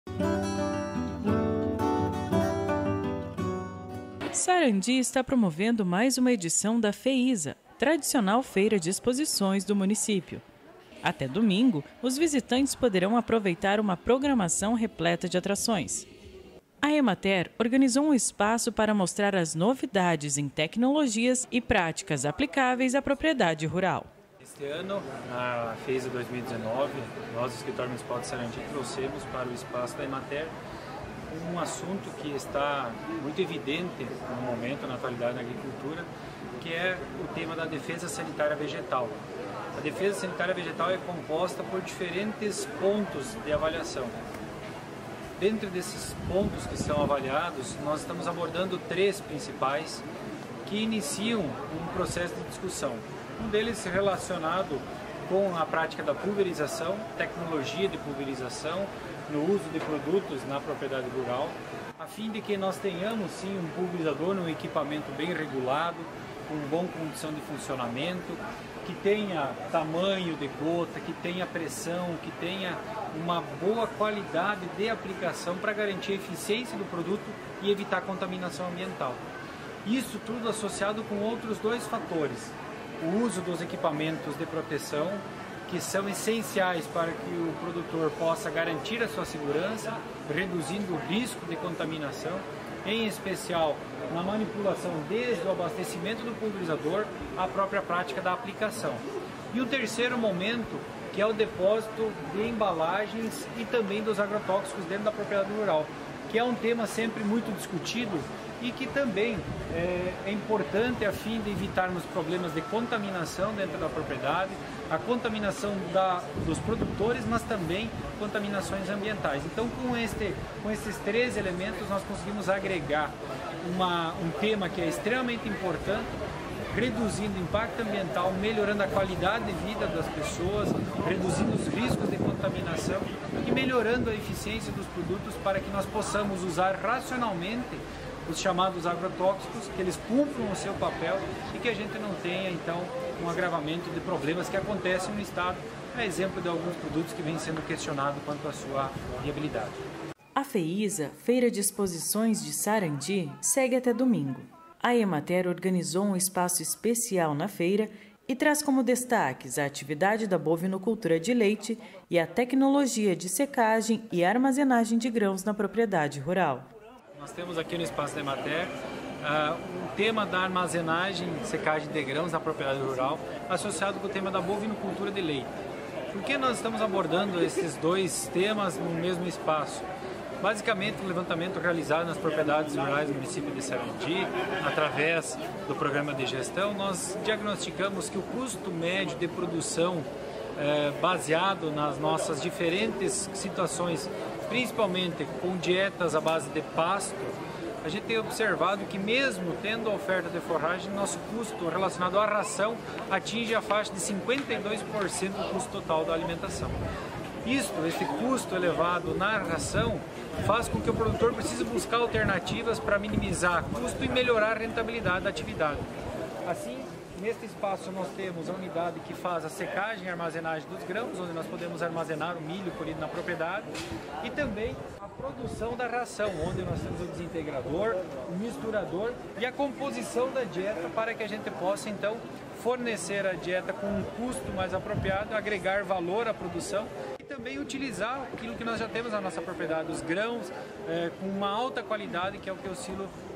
Tecnologia de secagem e armazenagem de grãos são destaques na FEISA - Rio Grande Rural
FEISA: Feira da Indústria de Sarandi, em Sarandi-RS

Programa #1065